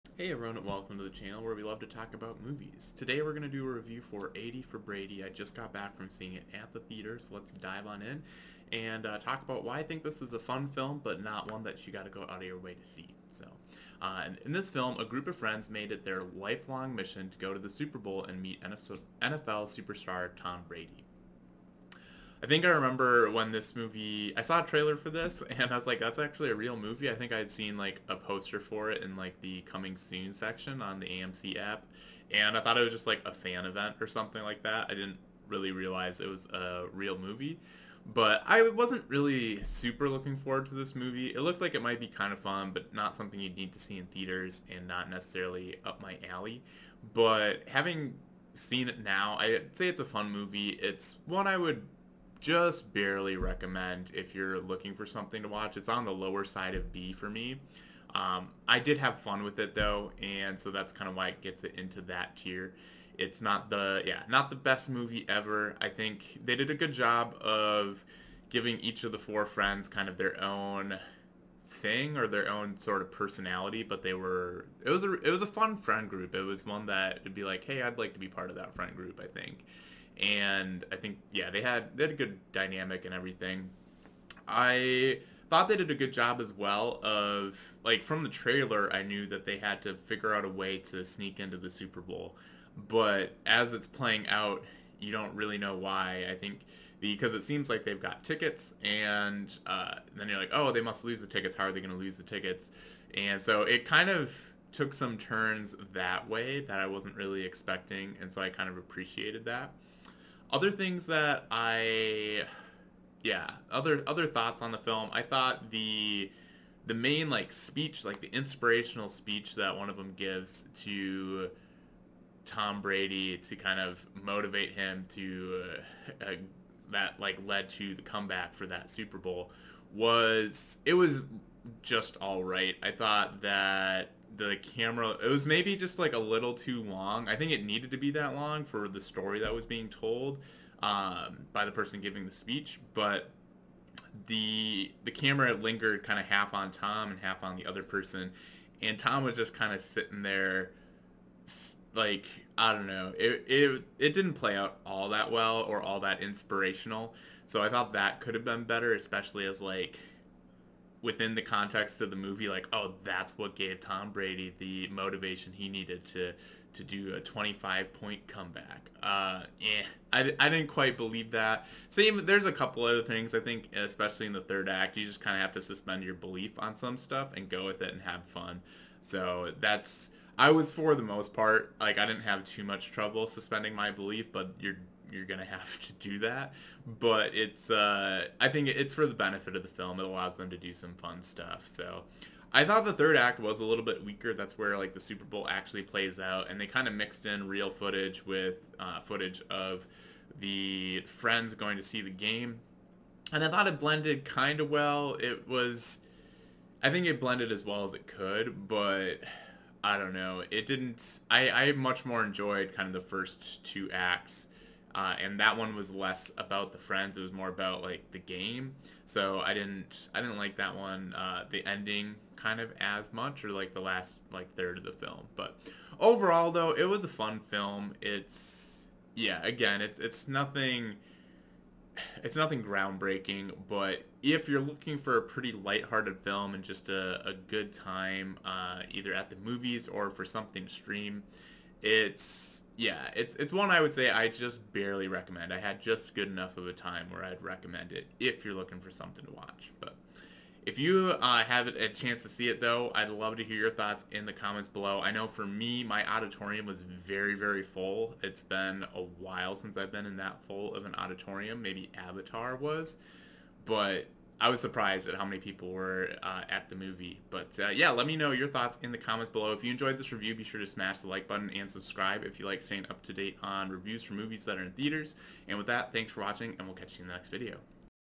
80 For Brady (2023) movie review - why I had a fun time with this movie
This is my review for 80 For Brady (2023) which was a fun little film! Share your thoughts on it in the comments below!